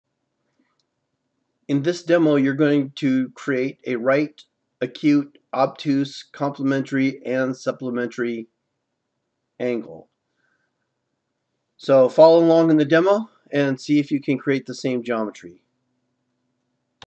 Creating Angles in AutoCAD
Online classes and virtual training found at the EvCC
This video was created to help students currently enrolled at EvCC
Demo: To demonstrate How to create a Lines & Identify Angles in the AutoCAD